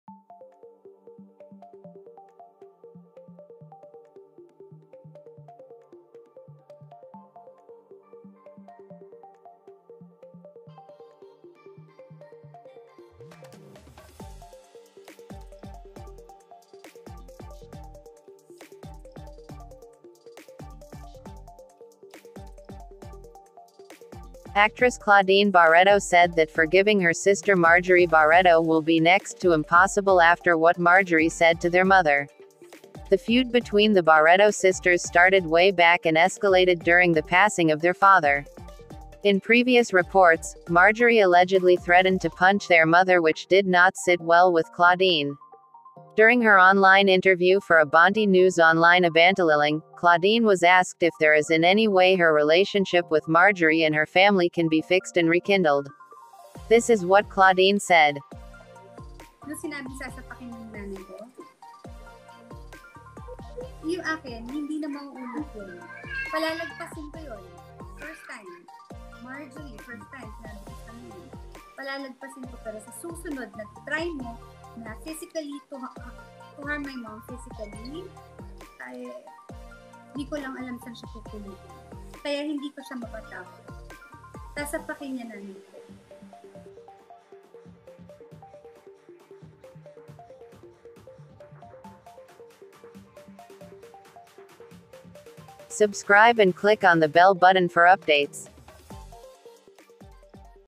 Claudine Barretto hindi na kayang patawarin pa si Marjorie Barretto
Actress Claudine Barretto said it is almost impossible that she can forgive sister Marjorie Barretto for what she did.

(Source:  FB - @Abante News Online / IG - @claubarretto / IG - @marjbarretto